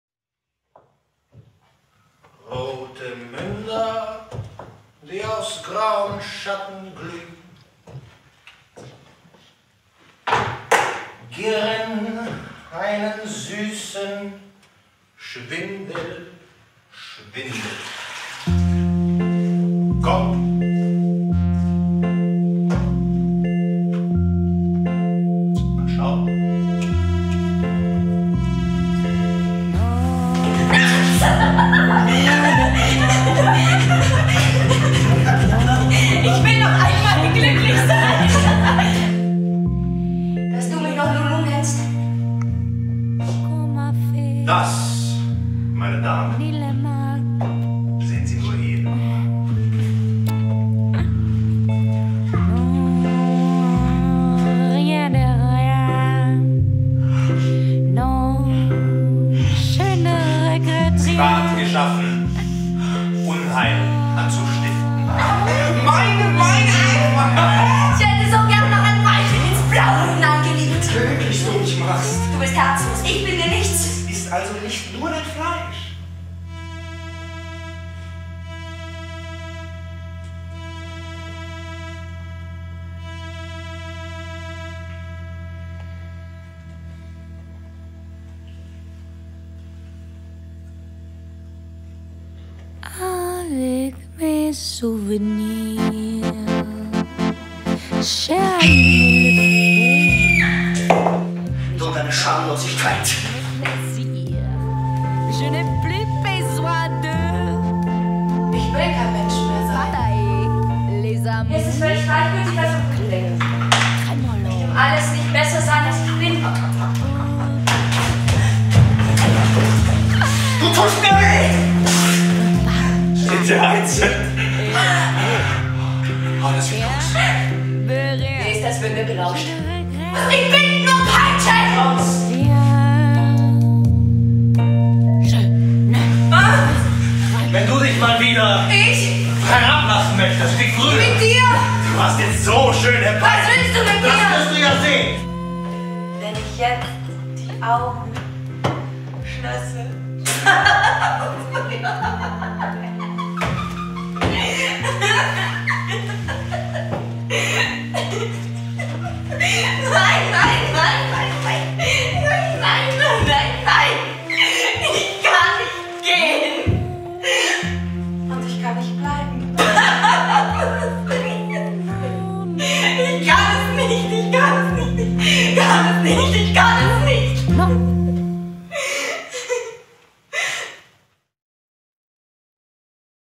Lulu Trailer David Stöhr Max Reinhardt Seminar
Und als Lulu hinausstieg und der flimmerndsten Blume gleich blühte, verloren wir uns für einen Augenblick. Es hätte eine gute Zeit werden sollen, doch während uns ihr erstaunlicher Gedanke dämmerte, graute uns Böses. Wie amüsiert wir also waren, als der Himmel sich verpflichtete auf sie herabzufallen. Da lag sie dann, zwischen den Beinen nichts sagend, ein Loch nunmehr in unserer Erinnerung. Endlich wähnten wir uns wieder in Eintracht. Und nur die Erde lachte leise: Ich will kein Mensch mehr sein.

mit Pauline Fusban, Marie-Luise Stockinger, Stefan Gorski, Lennart Lemster, Samouil Stoyanov, Andrei Viorel Tacu, Luka Vlatkovic und Lukas Watzl